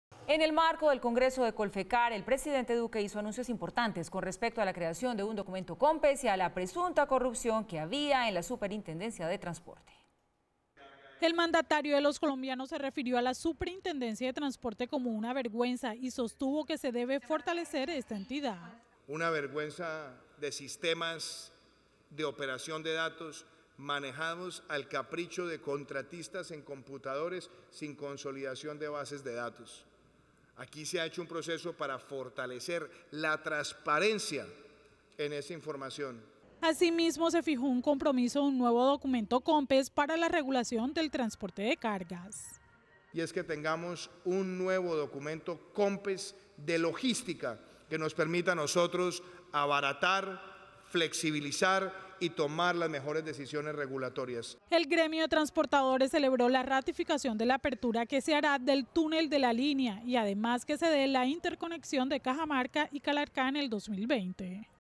Red+ | "La Superintendencia de Transporte, les quiero decir, es una vergüenza": Iván Duque
En el marco del congreso de Colfecar el presidente duque hizo anuncios importantes con respecto a la creación de un documento Conpes y a la presunta corrupción que había en la superintendencia de transporte